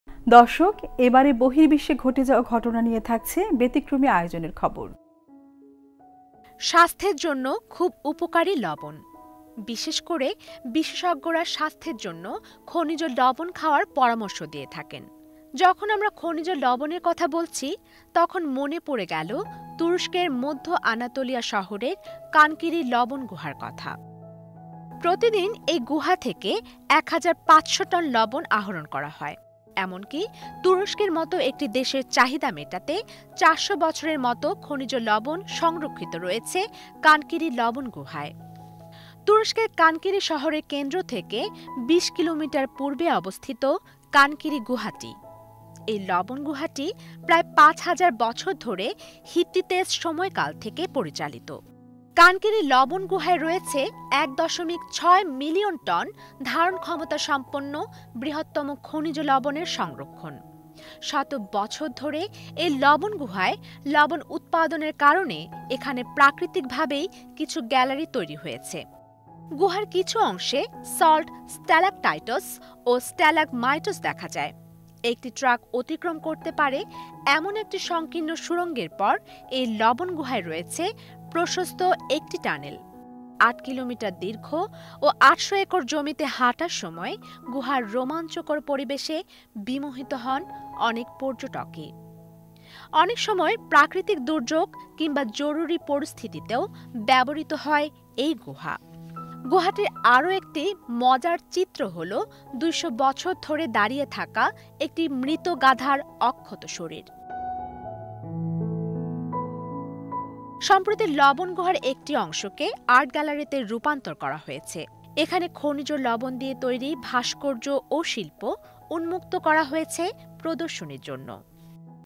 তুরস্কের লবণ গুহা (Salt Cave Turkey)
Cankiri Salt Cave.1500 tons of salt a day is expulse from the cave  and there is rock salt reserve enough for 400 years for Turkey in this rock salt cave.The cave is located 20 kilometres to the  east  part of the Cankiri city center . It is a salt cave which operated  since the Hittites period , for almost 5000 years . The cave has the largest rock salt reserves  with 1.6 million tons  capacity.
              As a result of  production for centuries ; many galleries naturally formed in the cave . salt stalactites and stalagmites can also be seen in some parts of the cave.After a narrow entrance where a truck can only pass you will encounter with wide tunnels  resemble a road tunnel . Visitors vill be excited from the great atmospher of the cave while they walk 8 kilometres long  and 800 acre land.  Cave is also can be used in case of any emergency as a natural , solid shelter.
        One of the other interesting images of the cave is the intact body of a dead donkey which was not deteriorated for 200 hundred years.  Due to the dry air available inside of the cave , the corpse of the donkey was stood intact like a mummified body.
         Recent years , one part of the salt cave was converted to an art gallery , and many statues and artworks made from the rock salts are exhibited in the cave and opened for visitors.